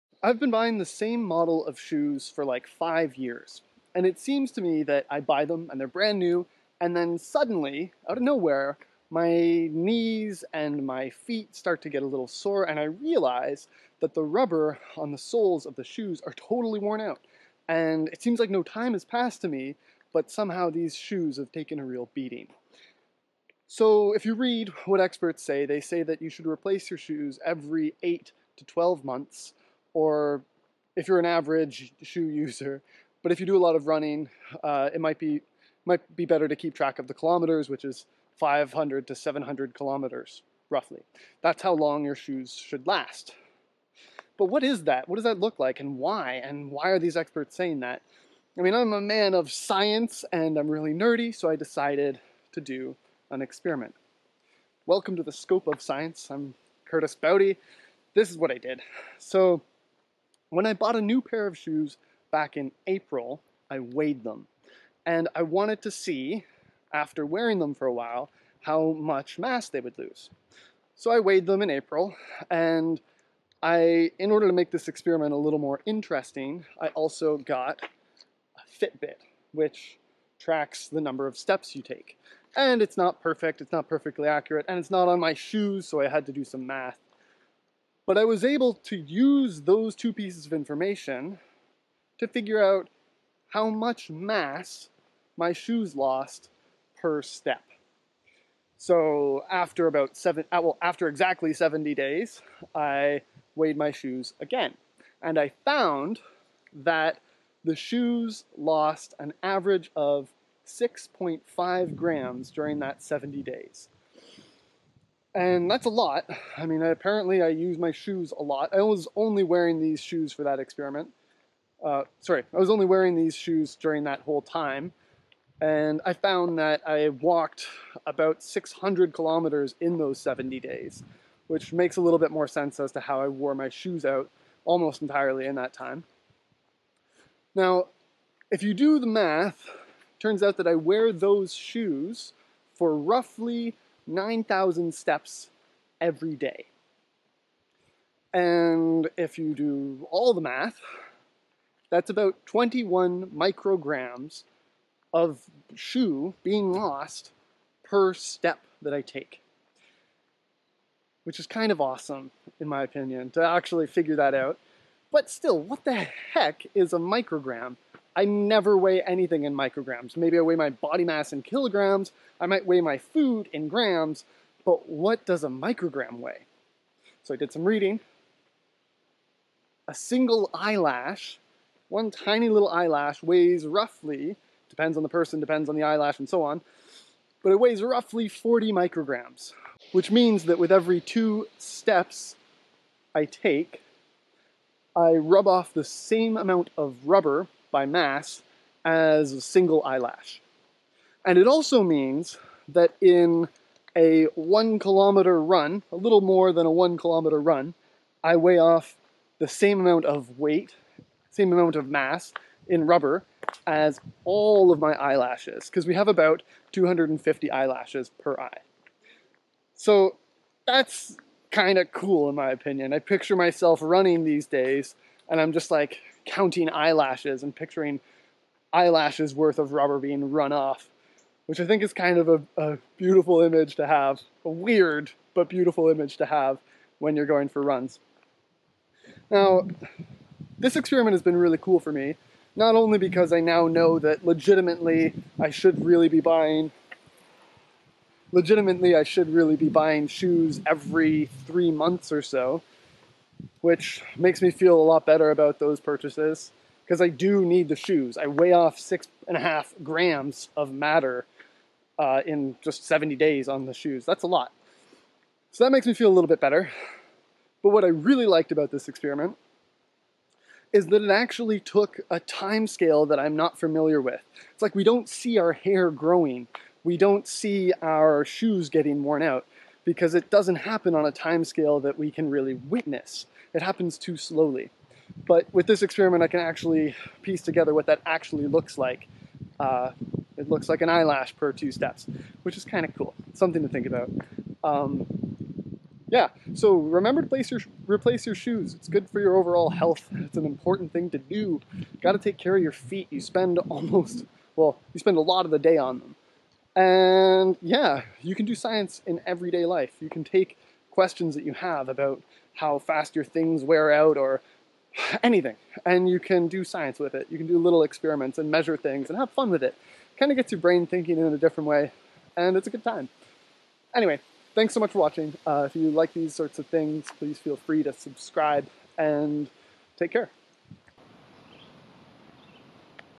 Exactly How Fast Do Shoes Get Worn Out?
I did something really dorky with my shoes. I wanted to know precisely how much rubber I burn when I walk, and how fast the soles of my shoes got worn away, so I did an experiment!